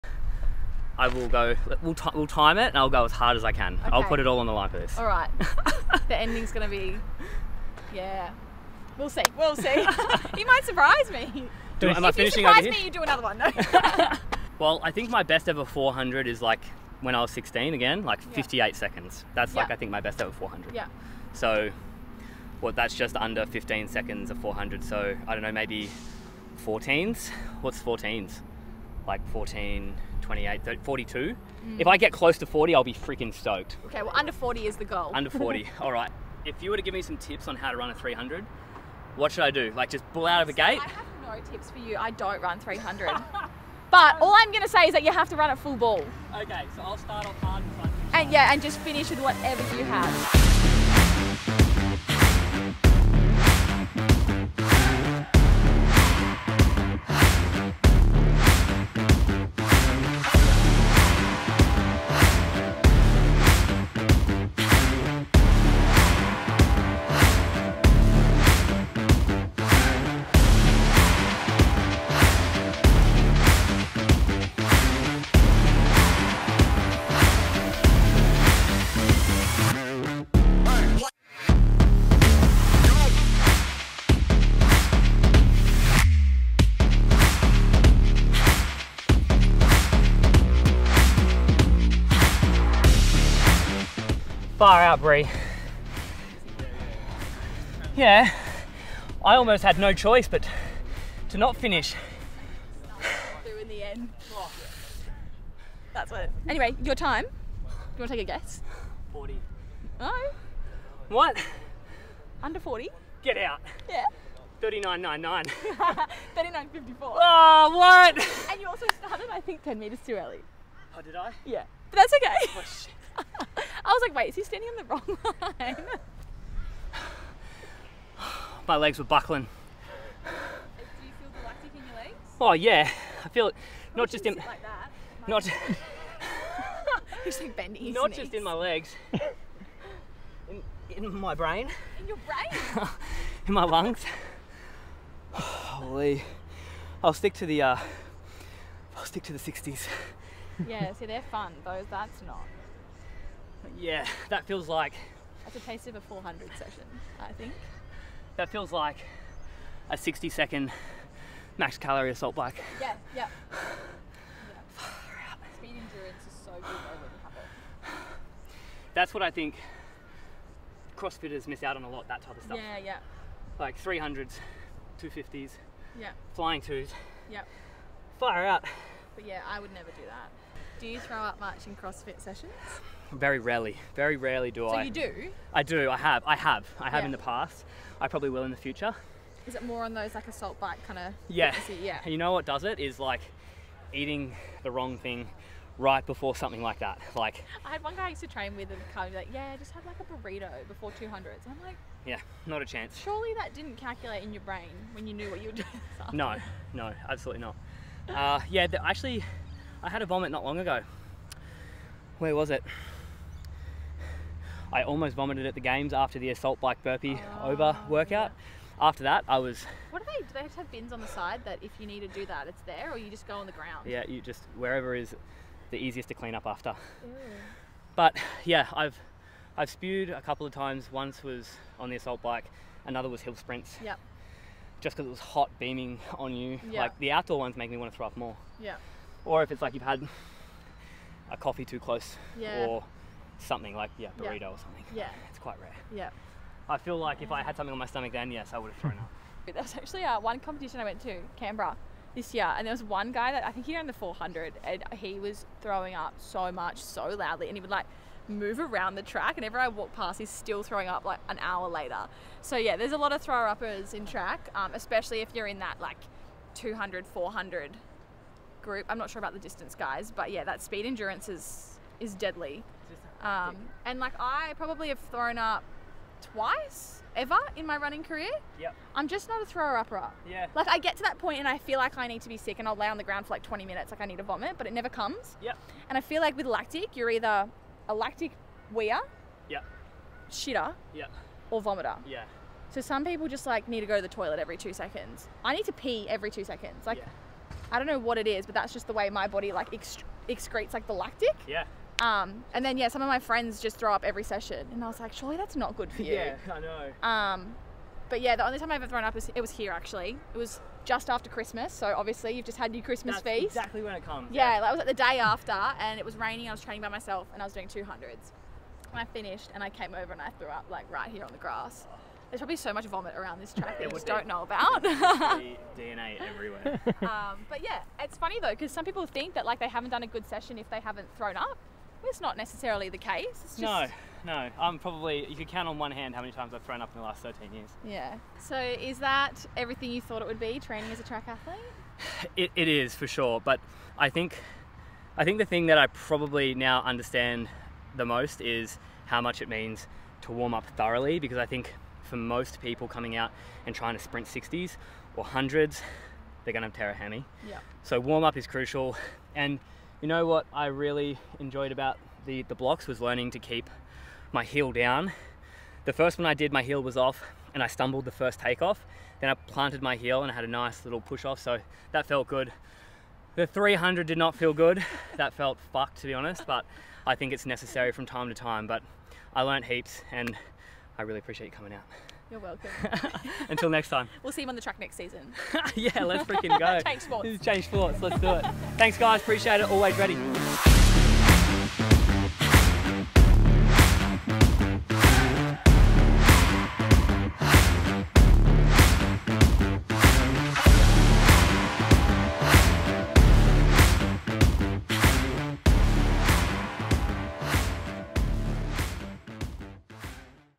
ALWAYS READY: Episode 2 - PART 2 CrossFit Champion vs. Olympic 100m Sprinter
THE CLIFF HANGER PART 2

ALWAYS READY: Episode 2 - CrossFit Champion vs. Olympic Sprinter Bree Masters

Join Phyba founder James Newbury in Episode 2 of ALWAYS READY, where he continues to push the boundaries of athletic performance. This time, James teams up with Australian Olympic 100m sprinter Bree Masters, a former beach sprint World Champion who transitioned to track in 2019. Bree quickly rose to the top, competing in the 2022 Oregon World Athletics Championships and the Birmingham Commonwealth Games.

In this episode:

Watch James, a 5-time CrossFit Games athlete, and Bree, an elite sprinter, tackle intense speed and endurance challenges
Dive into Bree’s journey from beach sprinting to becoming one of Australia's fastest women on the track
Learn the unique training methods and techniques of an Olympic sprinter and CrossFit champion
Explore how athletes adapt and excel in different sports disciplines
ALWAYS READY showcases peak human performance, with James proving that elite athletes can thrive on a plant-based diet powered by Phyba's plant-based protein and sports performance supplements.

Don’t miss this exciting episode as two world-class athletes from different arenas unite for an unforgettable test of speed, strength, and adaptability!